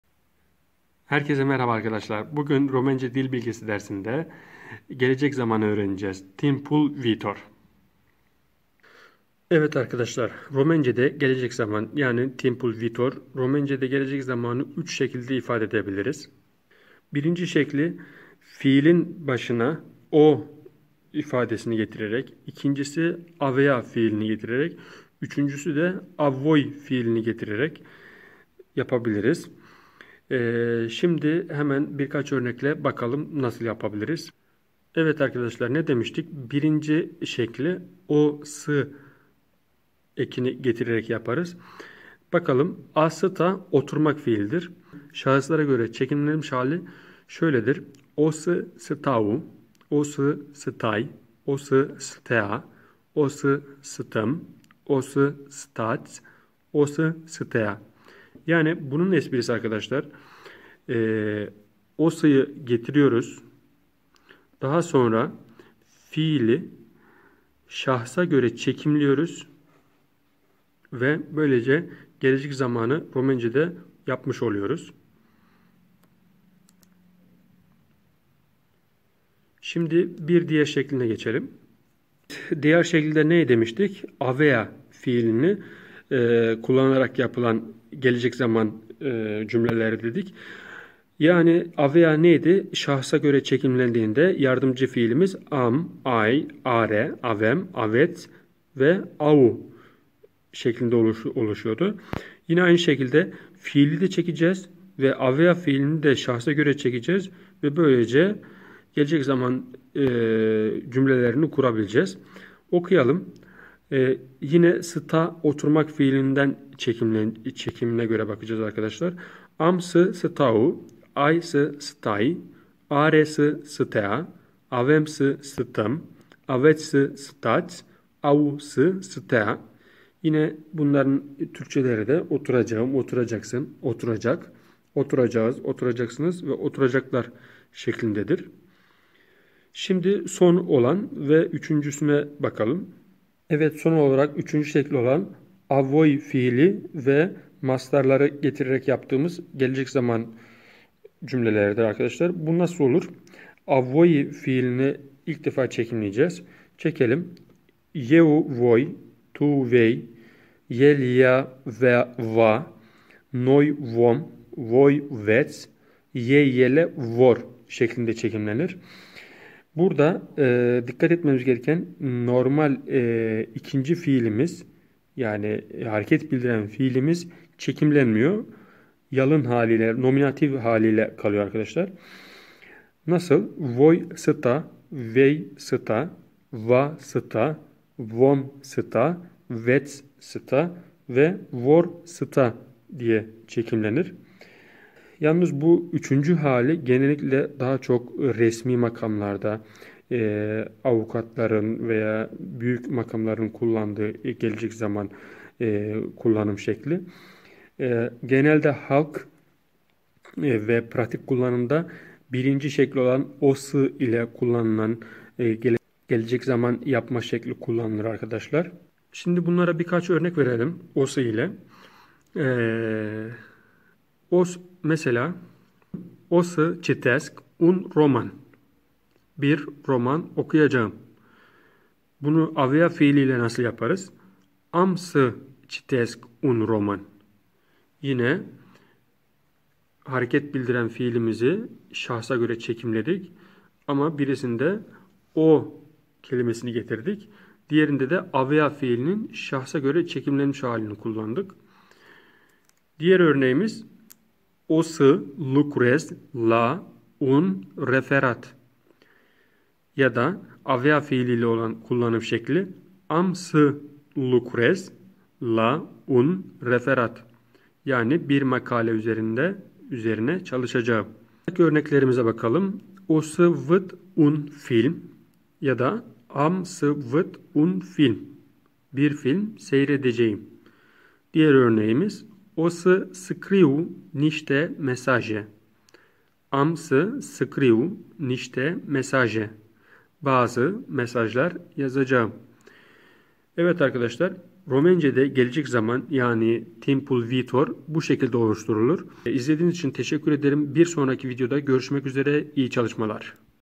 Ders 10 - ROMENCEDE GELECEK ZAMAN TİMPUL VİİTOR | ROMENCE ÖĞRENİYORUM
Bu dersimizde romencede gelecek zaman nasıl yapılır nasıl uygulanır örnekleri ile açıklamaya çalıştık ve öğrendik. Gelecek zamanın romence de 3 şekilde yapıldığını ilk formatın daha sık kullanılıdığını söyleyebilirim. Daha çok pratik yaparak daha kolay öğreneceğimiz gerçektir.

İyi çalışmalar!

Romence Öğreniyorum
Romence Dil Bilgisi
Romence Gramer
Rumence A1
romence konuşma
romence öğrenme